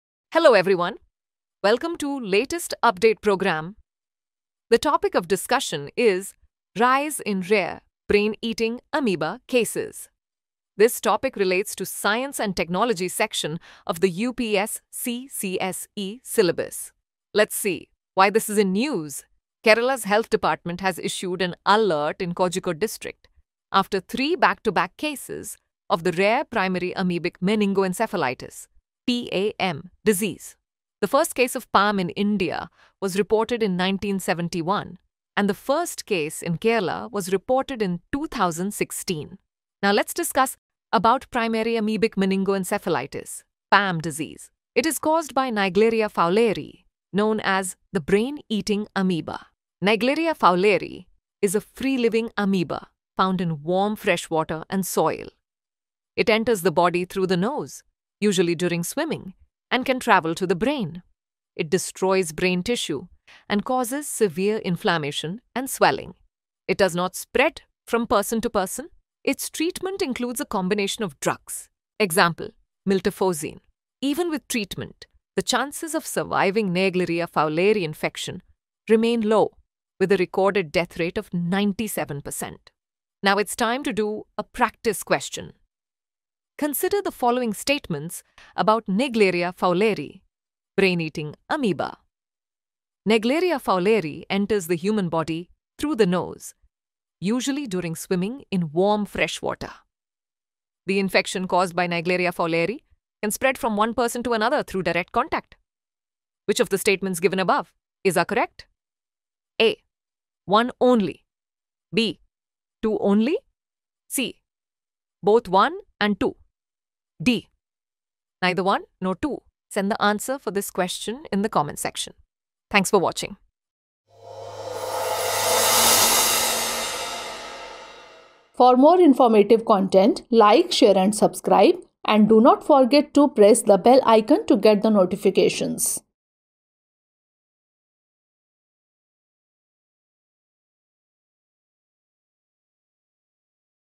Brain Eating Amoeba | Primary Amoebic Meningoencephalitis | Latest Update | Drishti IAS English
Kerala's health department has raised an alert in Kozhikode district after three cases of Primary Amoebic Meningoencephalitis (PAM), a rare disease caused by the "brain-eating amoeba" Naegleria fowleri. The first case of PAM in India was reported in 1971, with Kerala’s first case occurring in 2016. The recent rise in cases is attributed to factors such as improved testing for Acute Encephalitis Syndrome (AES), climate change, and environmental pollution.

Dear Viewers,